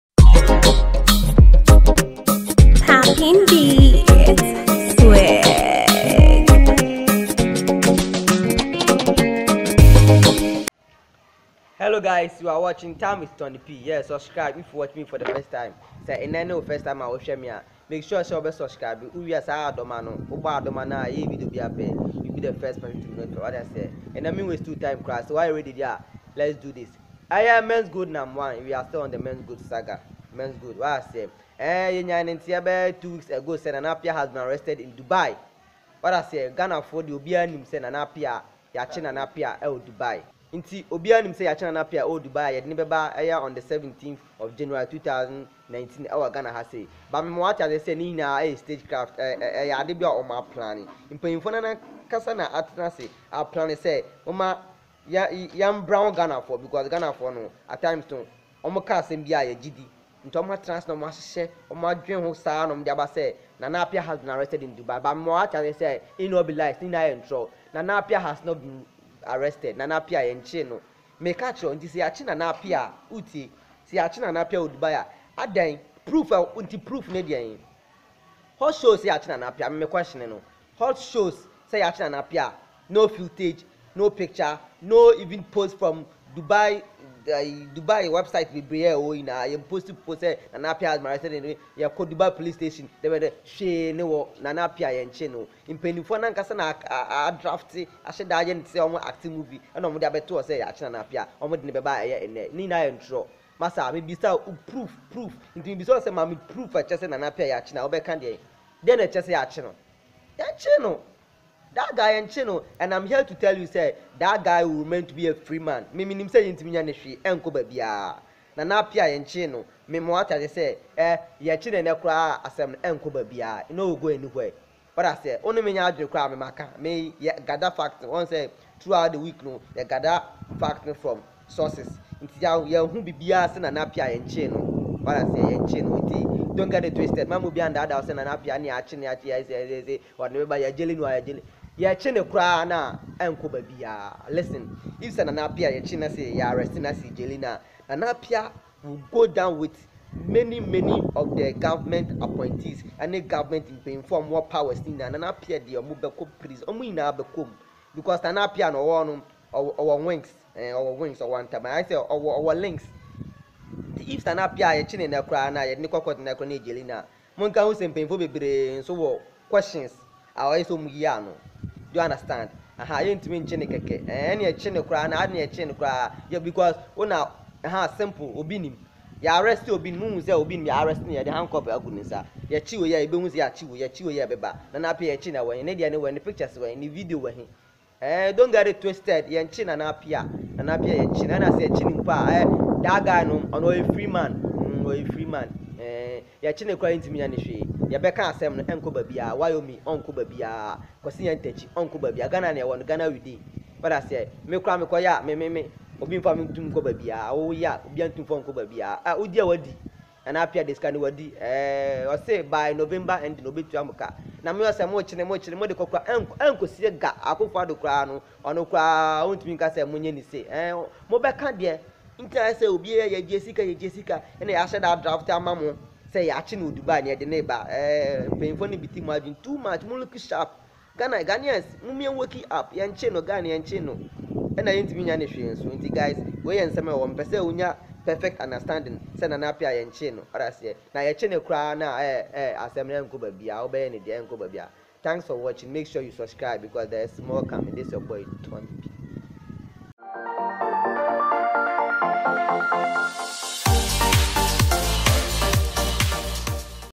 BREAKING! NAM 1 NOT ARRE$TED IN DUBAI JUST A COVER UP
CEO of Menzgold Nana Appiah Mensah popularly known as Nam1 who was reported to have been @rrested in Dubai a few days ago for defr@uding a man in Dubai has just been revealed to be a free man.